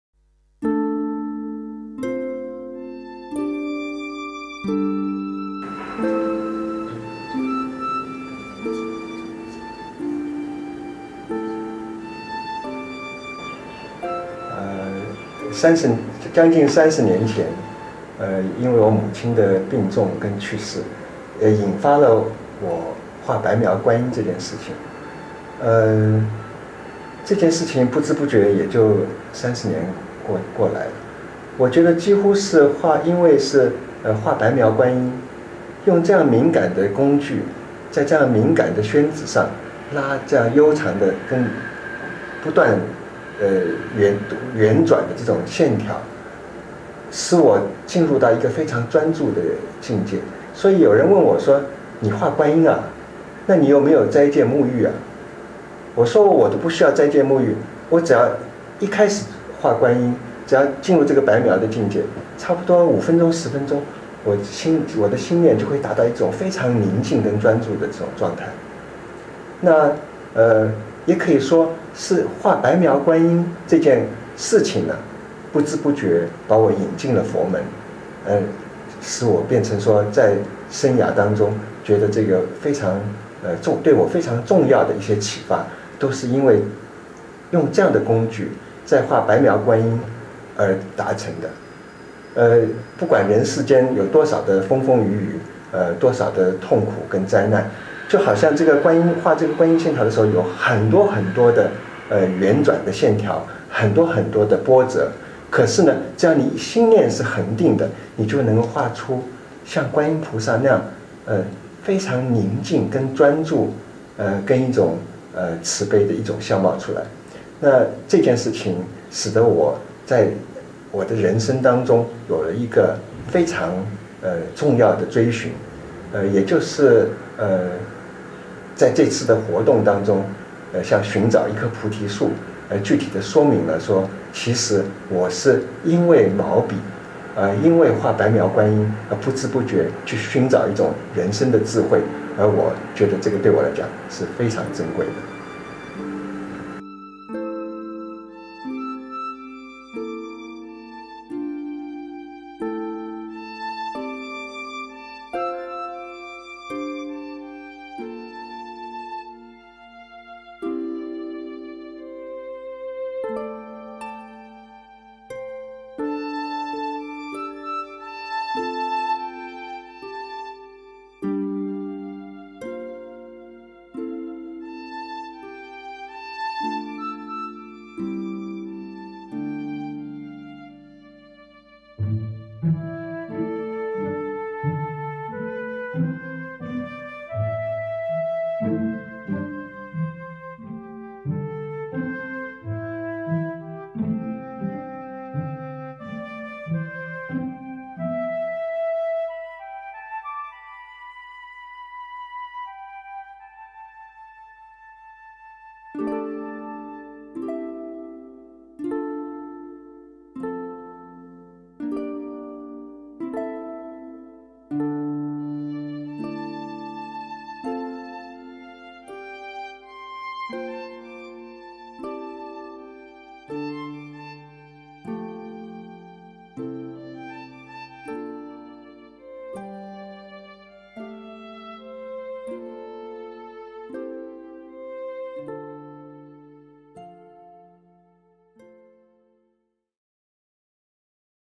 2010藝術家推廣教育系列『奚淞：白描篇』
奚淞多年來持續以筆墨、繪畫等手藝穩定自心；藉手牽引心，由專注、忘我，達到心靈的寧靜與解脫。奚淞希望把手藝經驗聚為「手藝禪」概念和內容，傳遞給所有試圖藉手創作穩定心靈的朋友。若你想更認識奚淞，請上